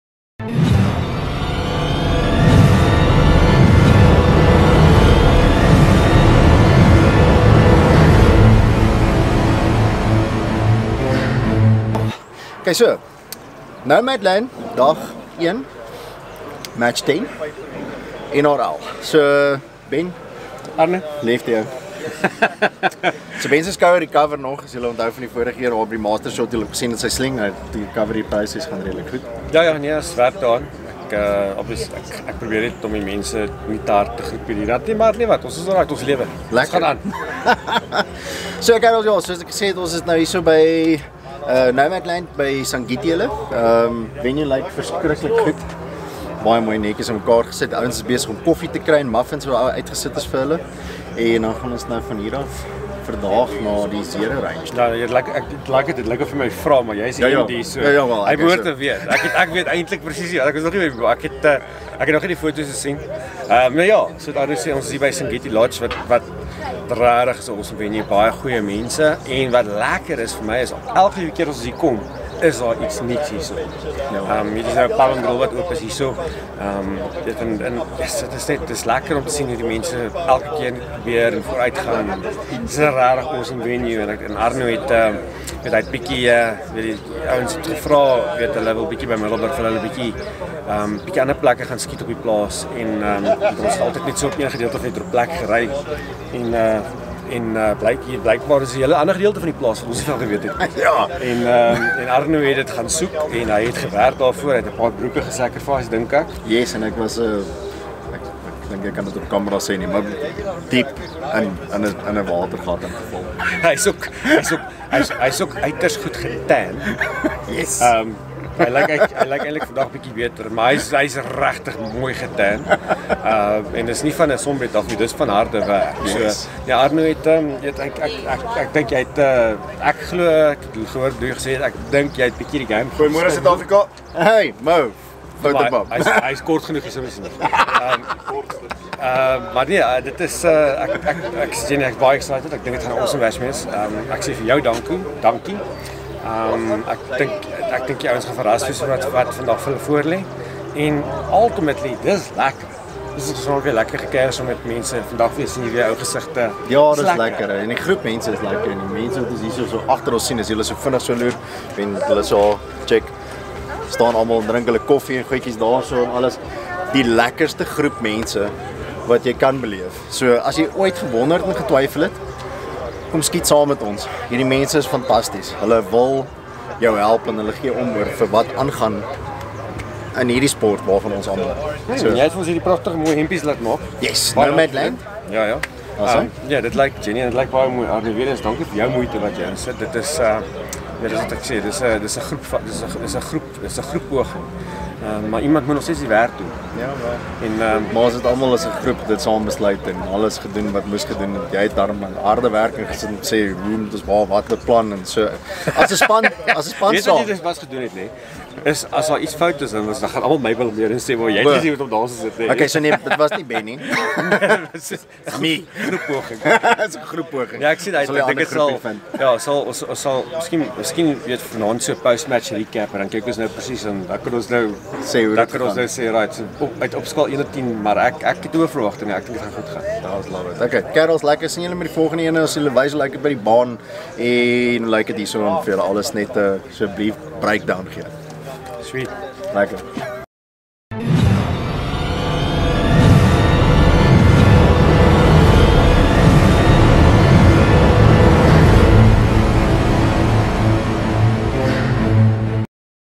NRL Nomadland Praat saam Ben
Ons gesels saam Ben by NRL Nomadland match 10 en 11